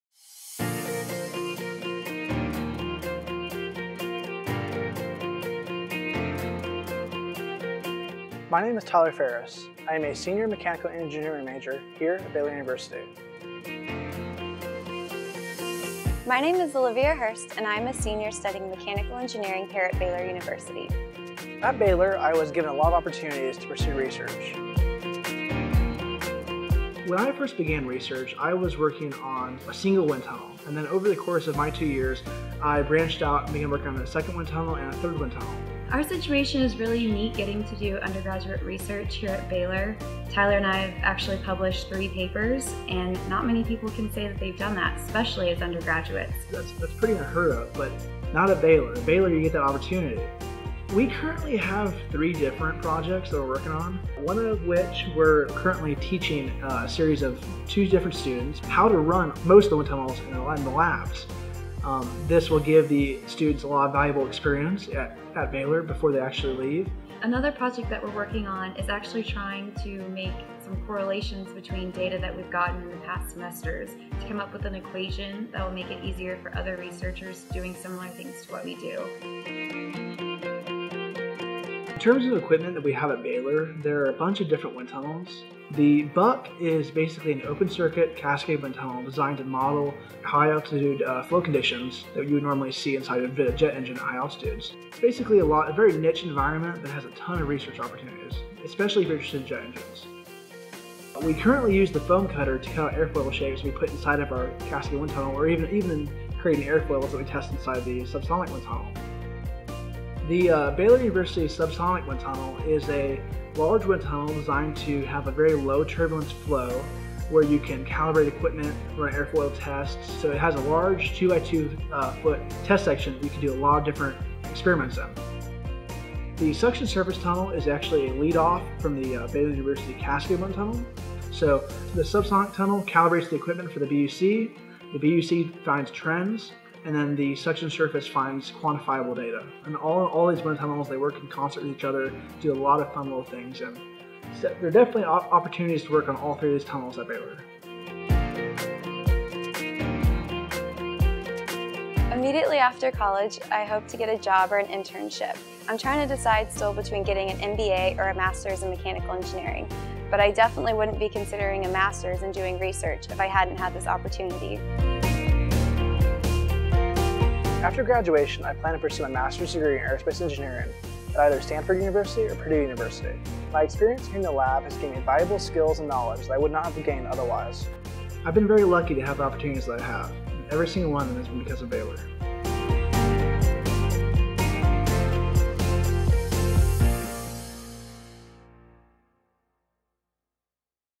Undergraduate Research at Baylor ECS
Our undergraduate students get a head start on participating in research projects that most students don't work on until graduate school. Watch this short video to learn about the research projects that two of our mechanical engineering seniors worked on as undergraduates at Baylor.